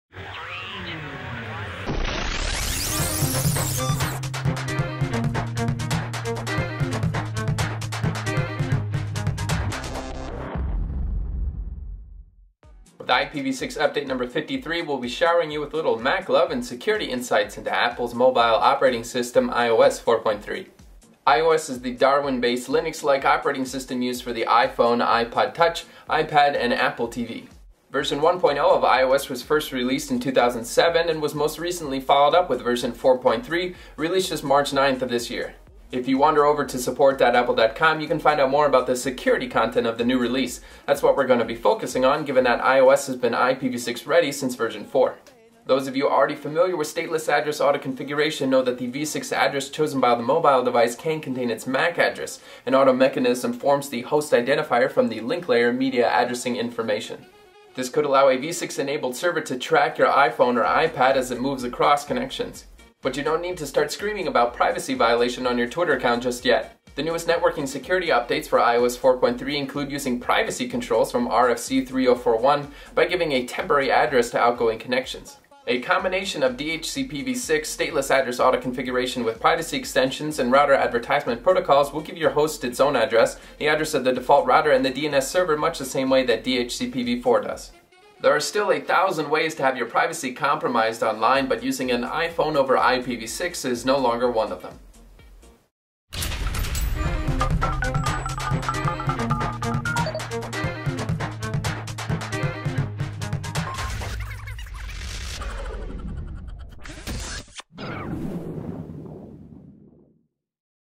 iOS 4.3 and IPv6: Privacy with the iPhone - he.net Webcast 53
Hurricane Electric Internet Services

Chris Van Fossen
Network Liaison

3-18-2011